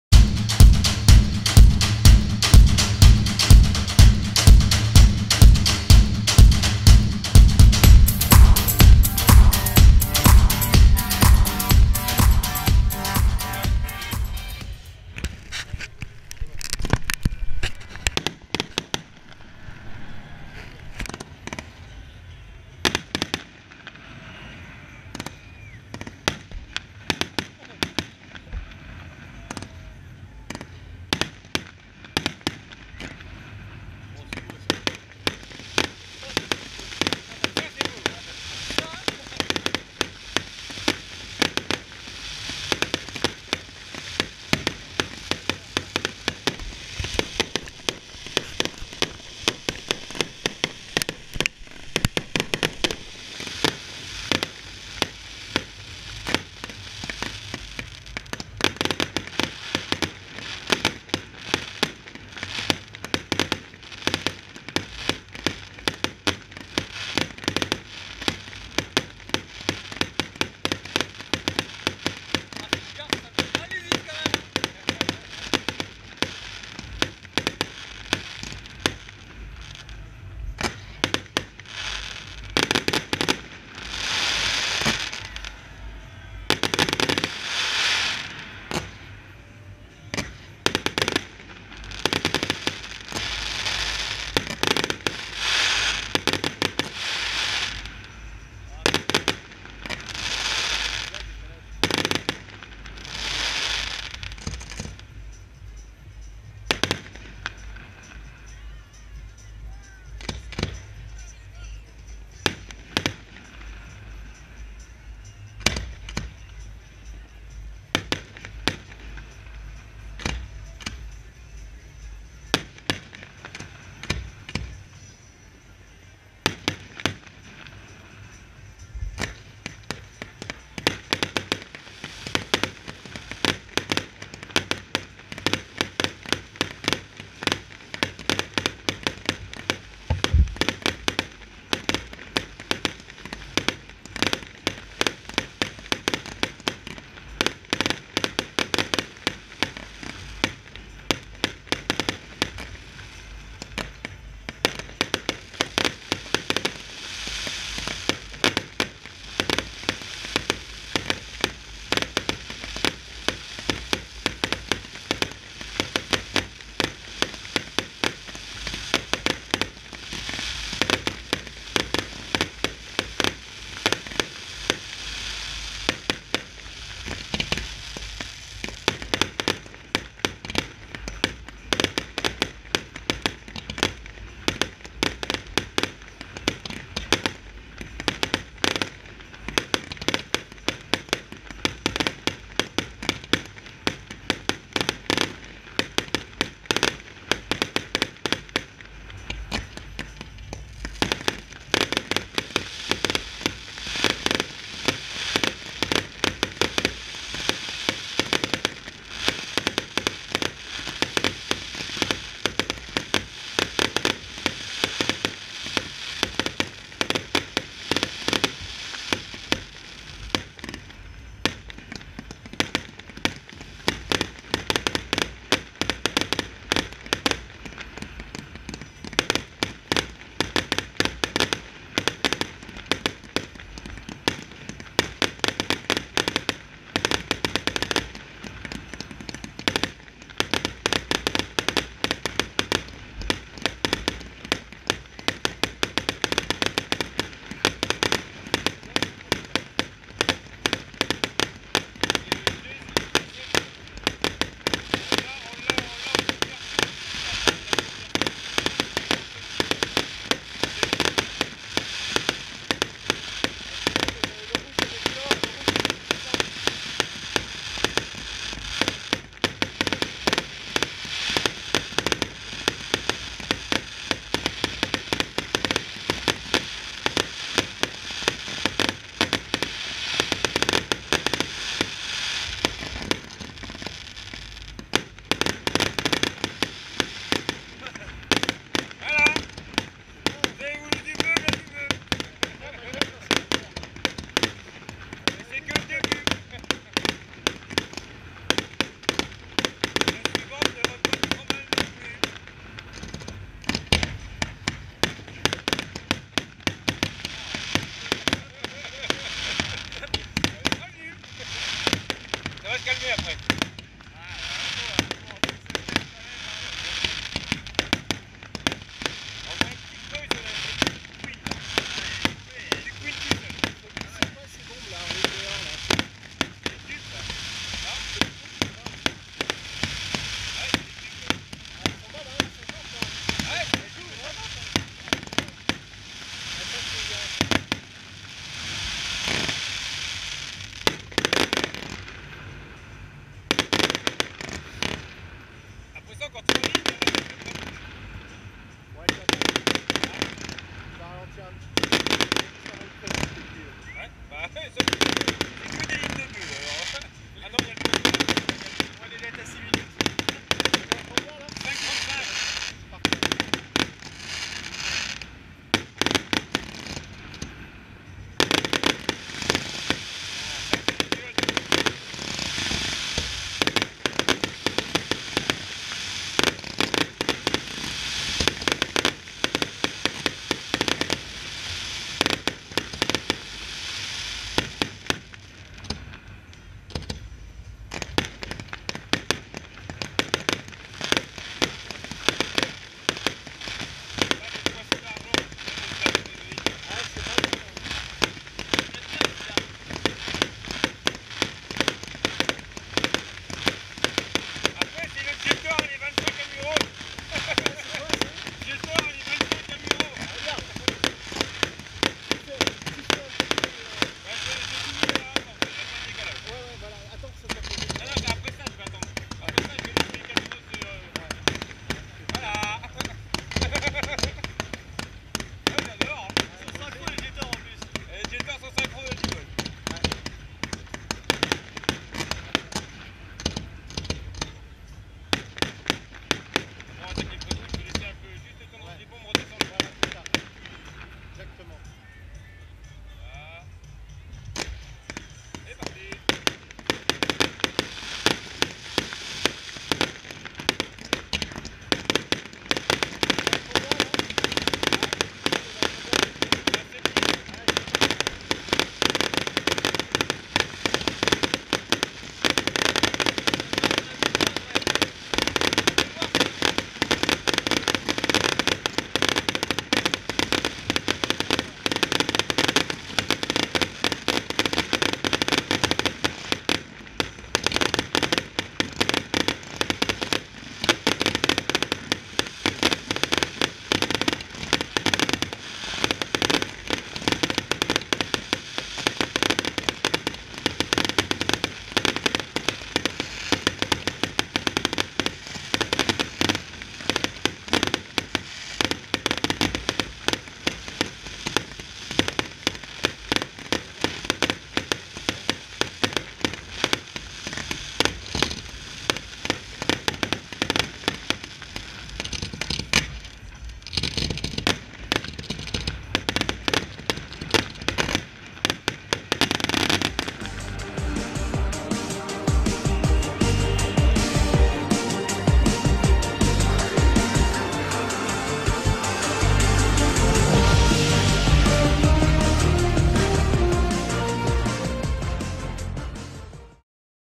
Feu d'Artifice - Carnaval Granville 2015 - Locatech Artifice
A partir d'une simple étincelle, nous ferons de votre fête une explosion de magie pyrotechnique.